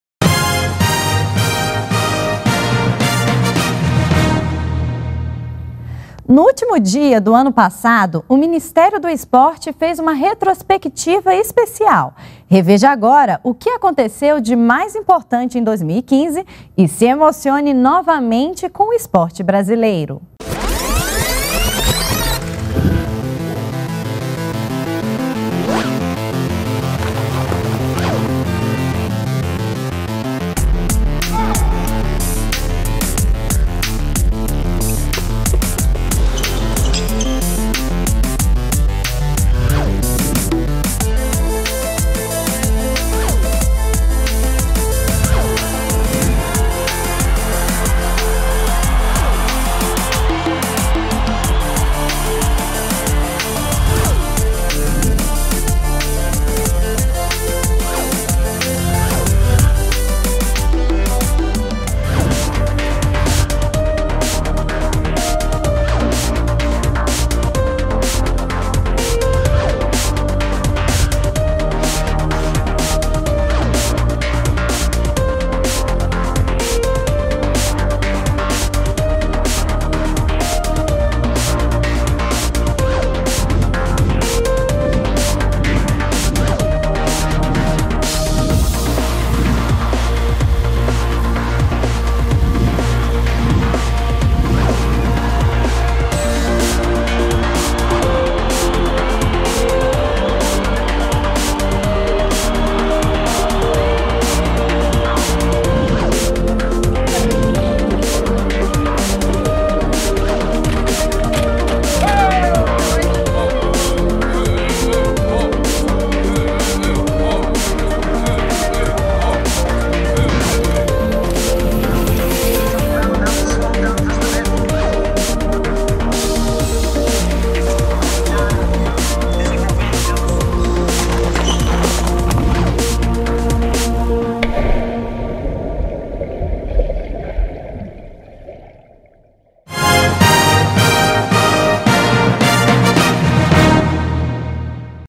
Esporte brasileiro emocionou atletas e sociedade em 2015
BRASIL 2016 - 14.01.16: Em 2015, o Ministério do Esporte fez uma retrospectiva especial sobre as emoções do esporte nacional.